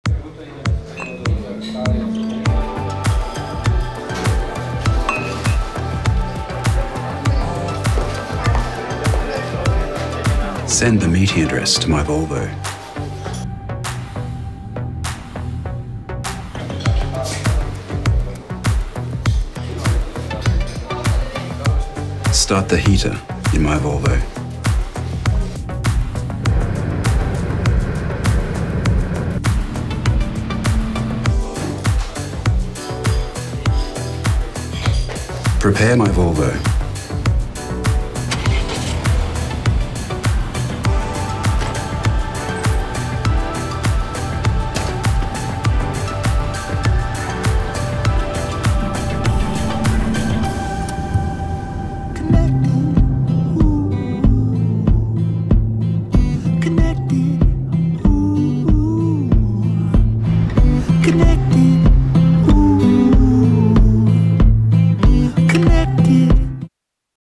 The Future Is Now: Volvo Cars And Microsoft Enable People To Talk To Their Cars
Introducing a first in automotive connectivity. Voice control on the Microsoft Band 2, announced at CES 2016.